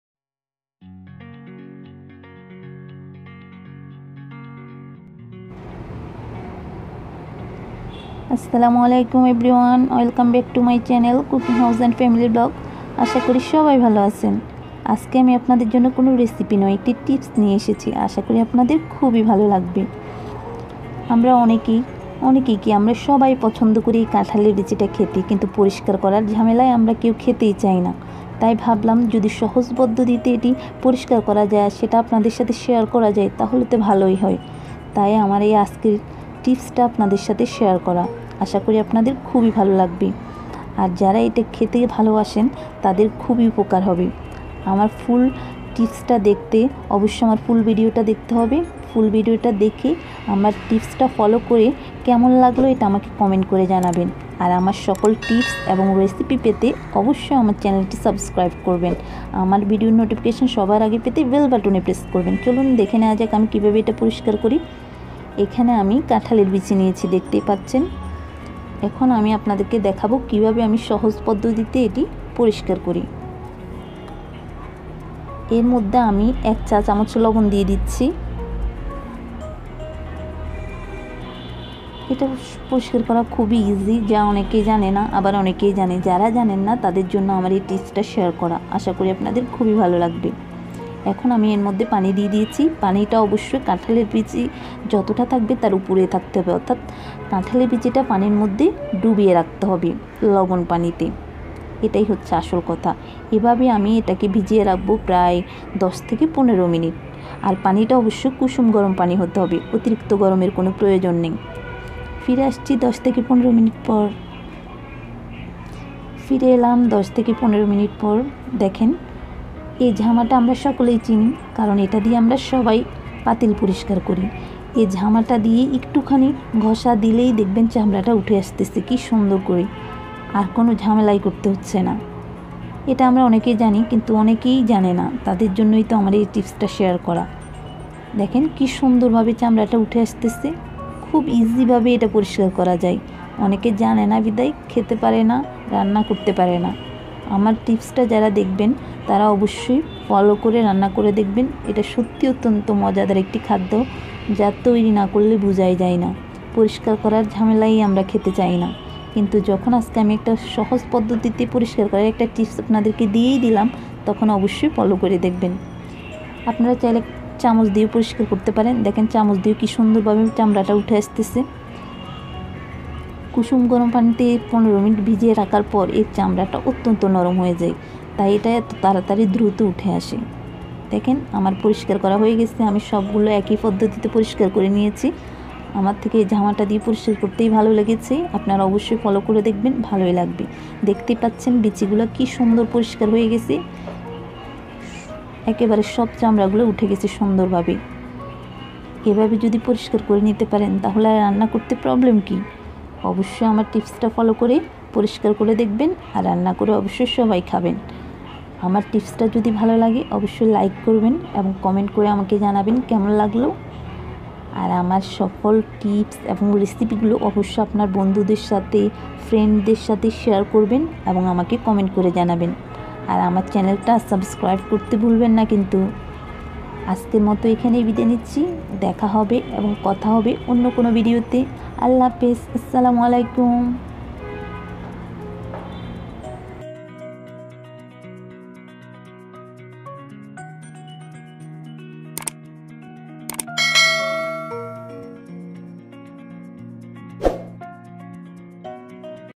কাঁঠালের বিচি পরিষ্কার করার খুবই সহজ একটি পদ্ধতি 👍👍🍴
আমার টিপসটি ফলো করে দেখবেন,আশা করছি আপনাদের কাজে লাগবে।।।
আর আমার ভিডিওর নোটিফিকেশন সবার আগে পেতে বেল বাটনে প্রেস করতে ভুলবেন না।।।।